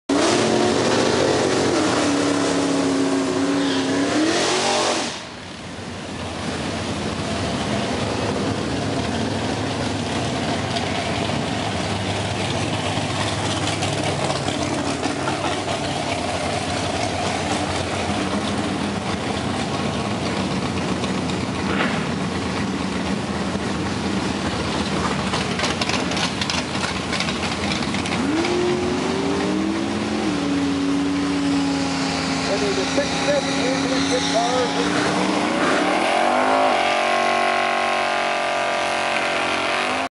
PRO-STREET MUSTANG COUPE * CHOPPED DROPPED TUBBED WHEELIE BARS CHUTE WING* NASTY * OMG
This was one crazy Mustang. It never did run at the dragstrip. I was really hoping to see it in action. It had 30-day tags on it.